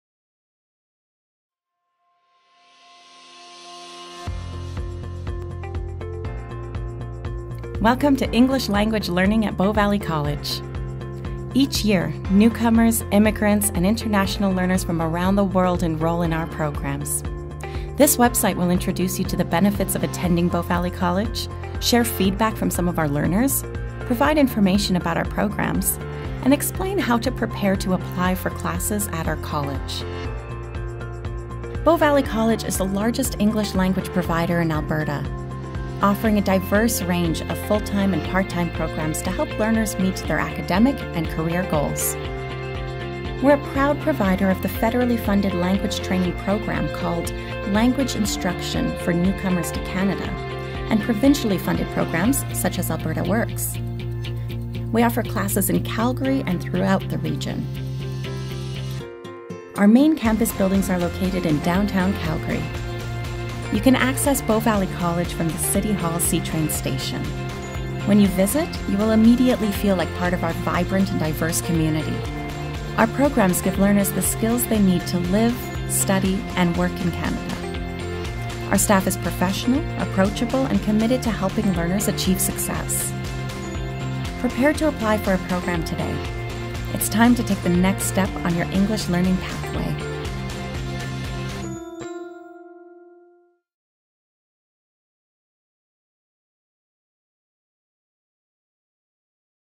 Welcome to English Language Learning at Bow Valley College
Bow Valley College is the largest English language provider in Alberta, offering a range of full-time and part-time programs to help learners meet their academic and career goals. Our programs help learners to live, study, and work in Canada.

For more information in multiple languages about our ELL programs, visit Apply ELL